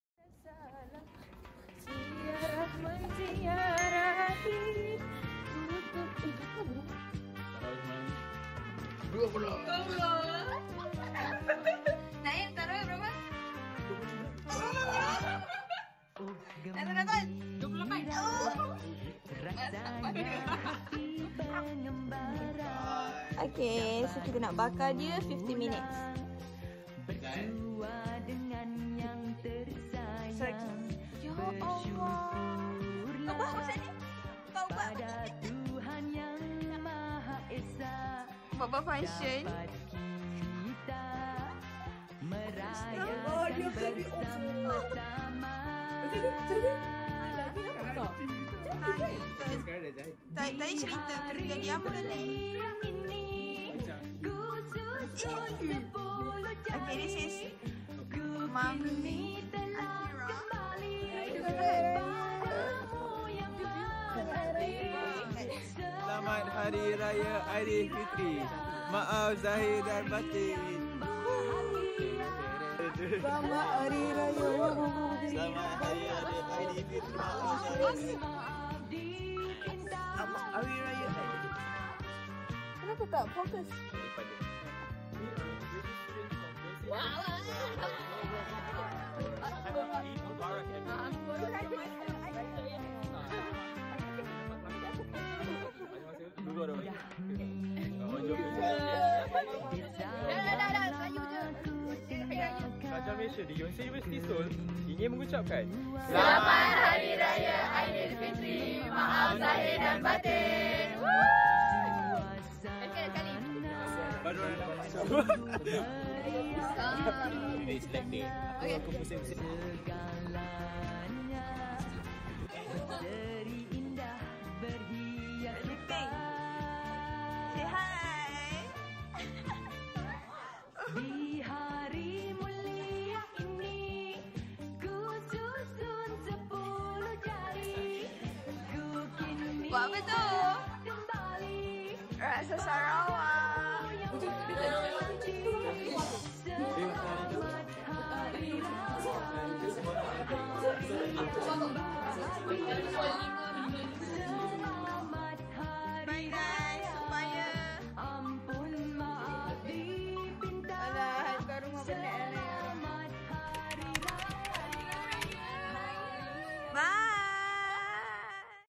CEREKA RAYA 2024 | Raya at Yonsei
Caption:
"Anak perantau, berlapar dahaga
Saban hari mengidam Gosami
Bunga sudah mekar, Syawal pun tiba
Selamat Hari Raya dari Yonsei University"

Created by:
ZURAIDA BINTI ZULKIFLI
B22
YONSEI UNIVERSITY